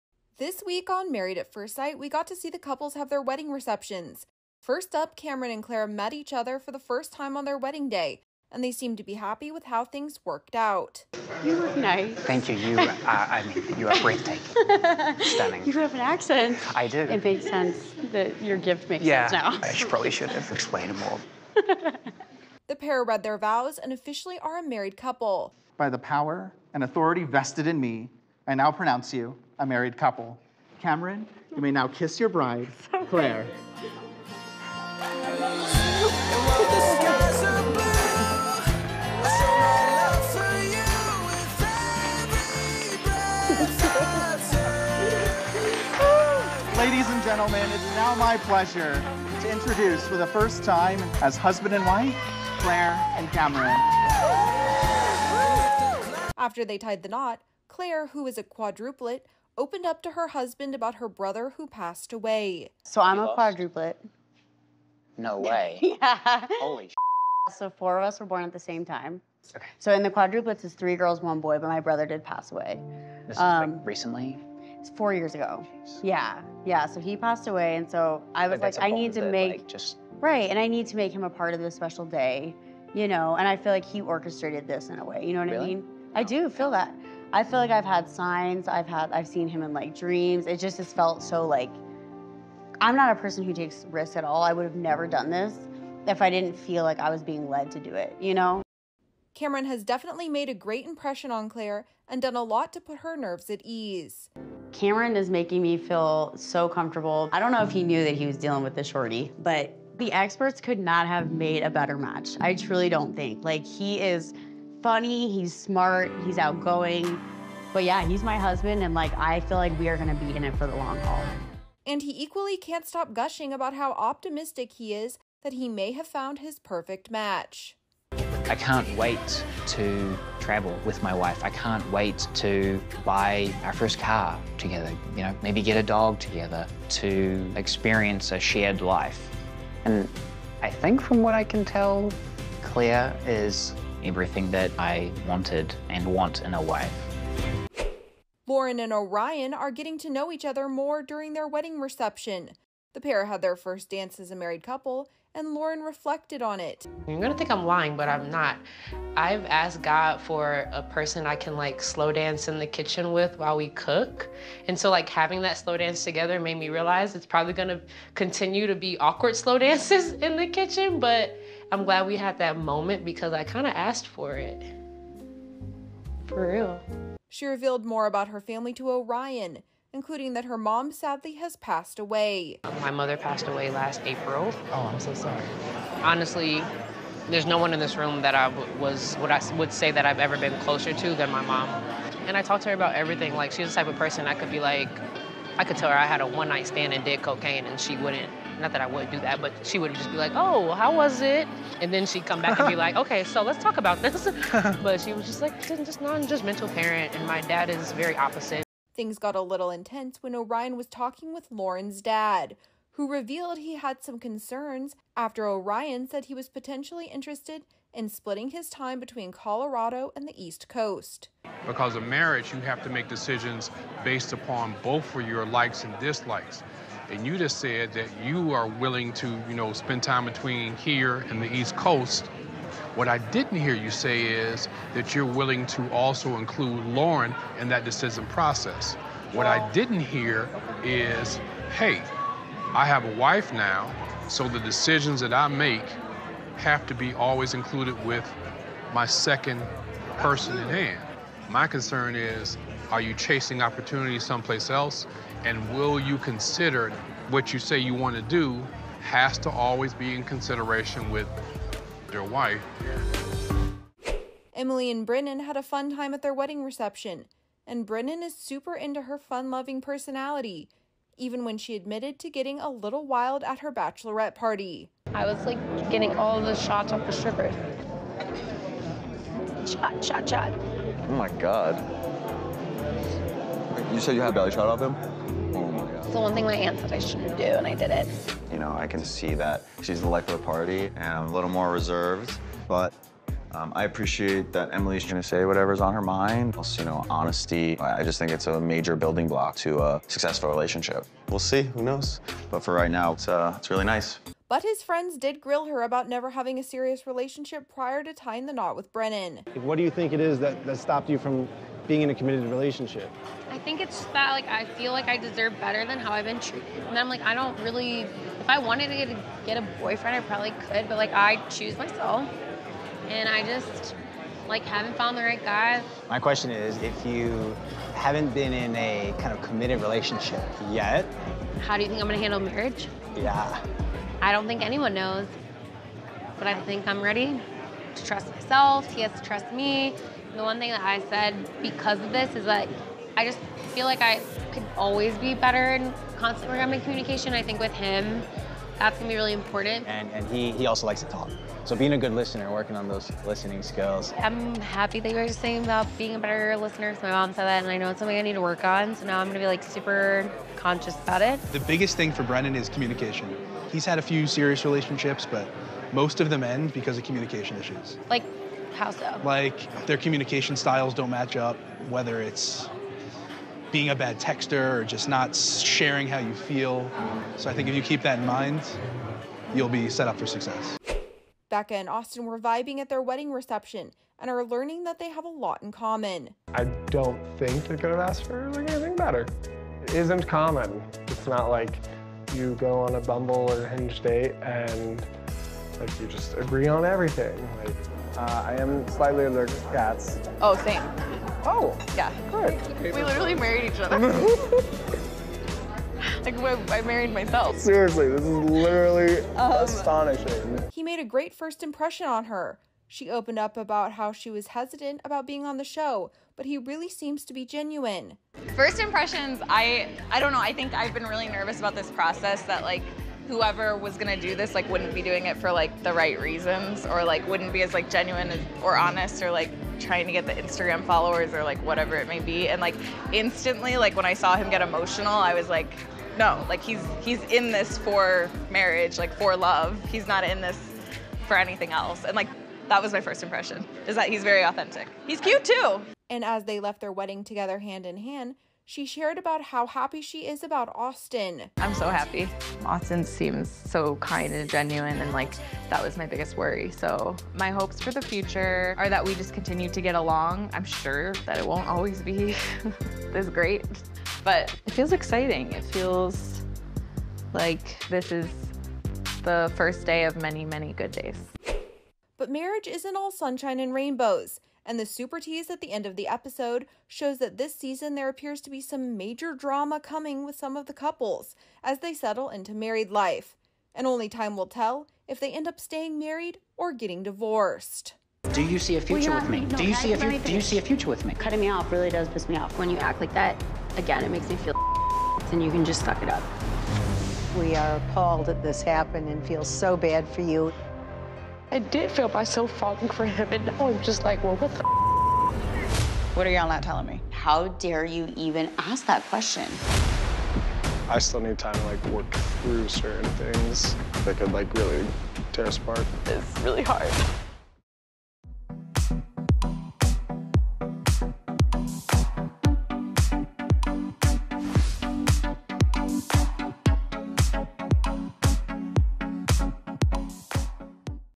‘Married At First Sight’: Cameron & Clare Smitten, Orion Gets Grilled
This week on “Married At First Sight,” we got to see the couples have their wedding receptions. From Clare and Cameron seemingly being smitten Orion have a tense moment while getting grilled by Lauren’s dad – here’s everything that went down. “Married At First Sight”- produced by Kinetic Content - airs Wednesdays at 8pm PT/ET on Lifetime.